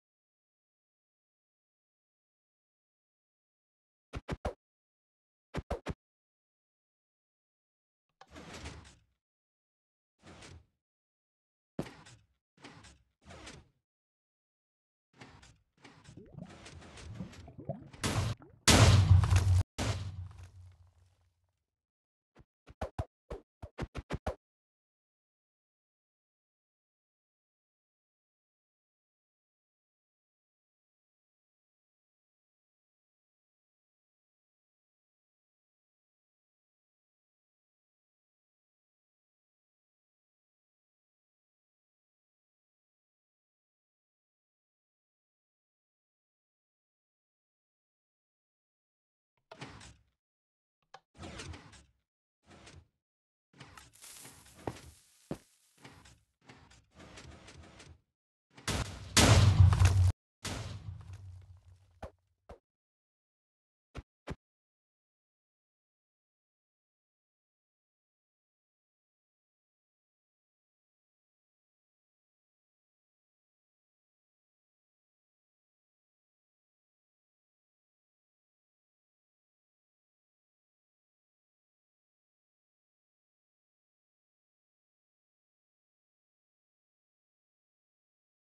Tnt launcher
about 100 TNT that shot me it to the space